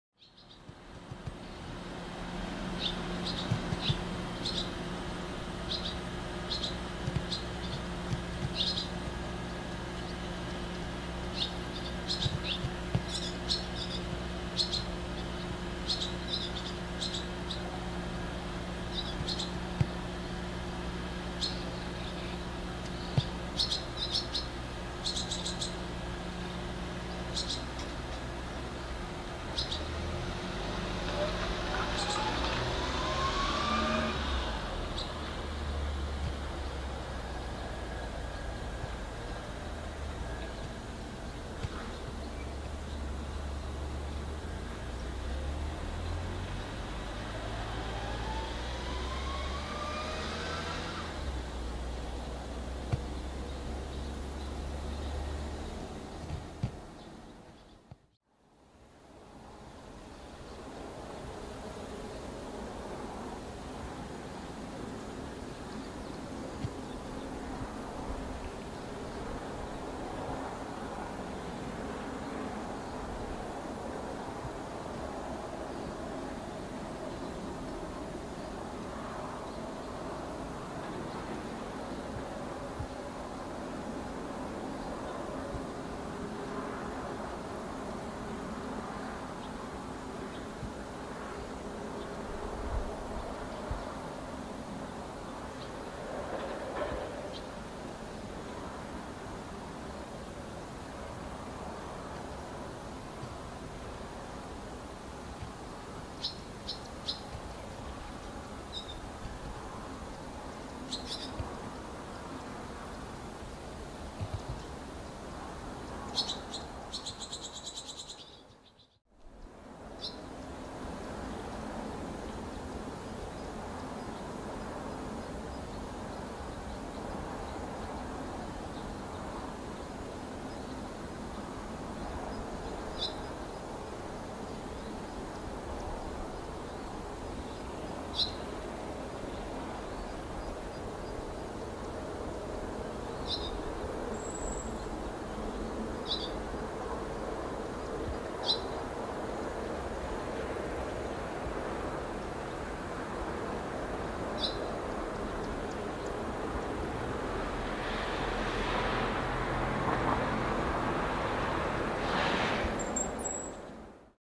Just enjoy watching cute Barn Swallows 🐧
Very young Barn Swallows (Tsubame) were perching on an electric cable. Let's enjoy their cuteness! 🐤